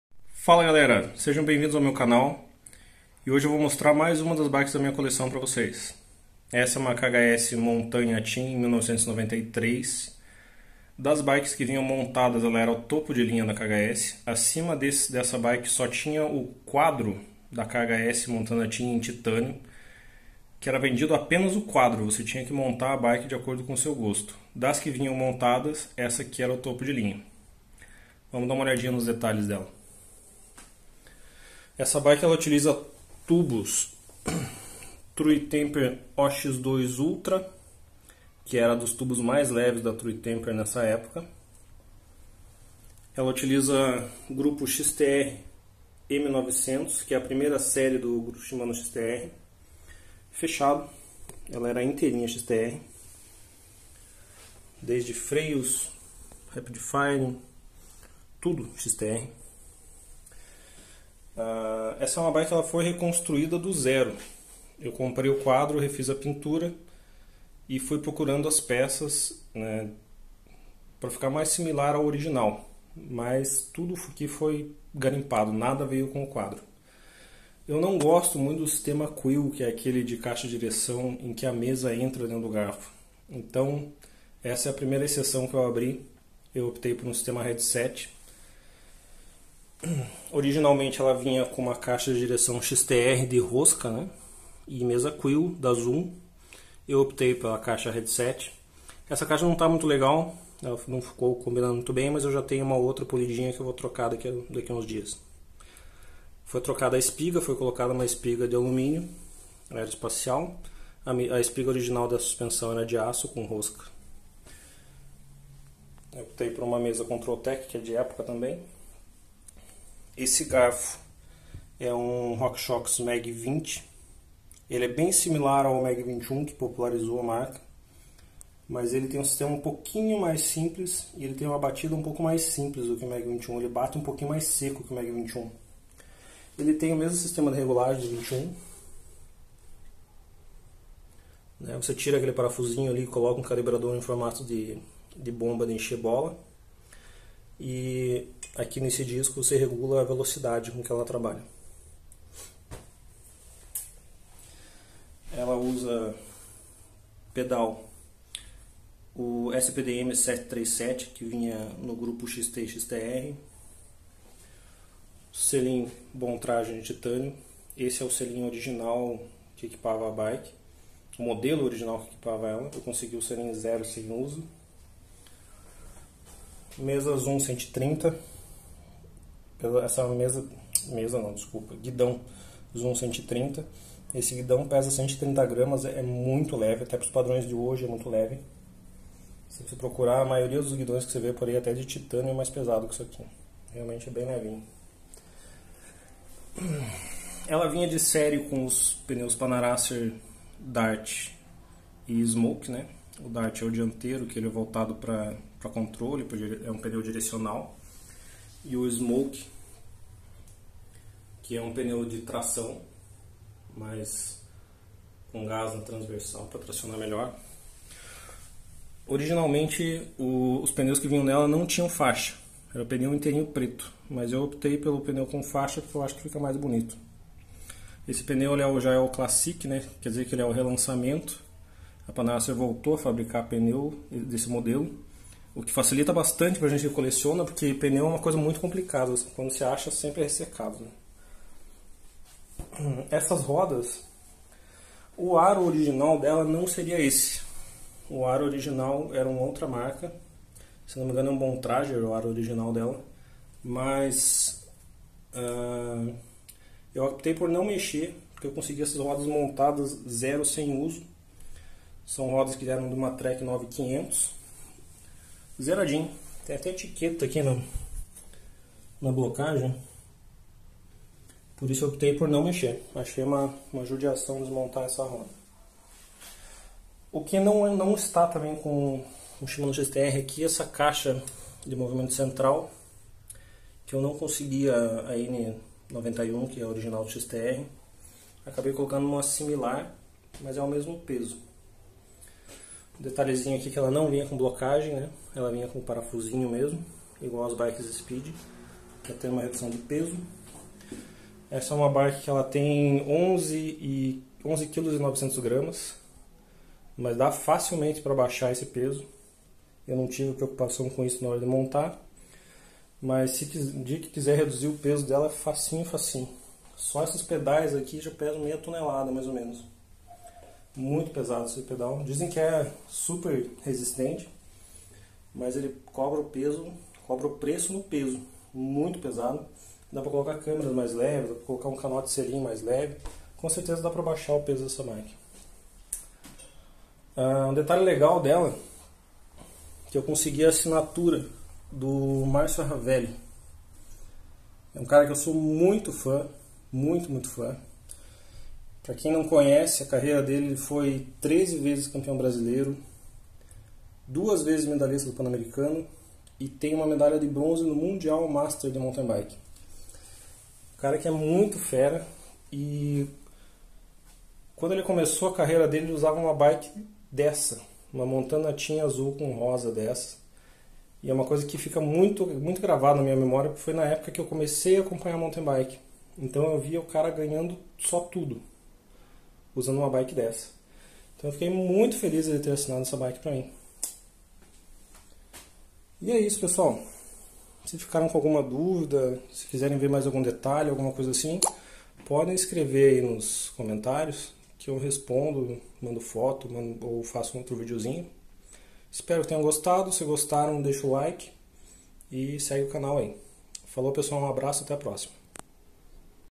Vintage MTB - KHS MONTANA TEAM 1993
KHS Montaña Team 1993, bike totalmente restaurada/reconstruída, no padrão de época, e assinada pelo multicampeão brasileiro Marcio Ravelli. Grupo Shimano XTR M900, garfo RockShox MAG20, pneus Panaracer Dart e Smoke e selim Brontrager em titânio.

A suspensão foi um trabalho de restauração do Jassa. (@jassa.biker).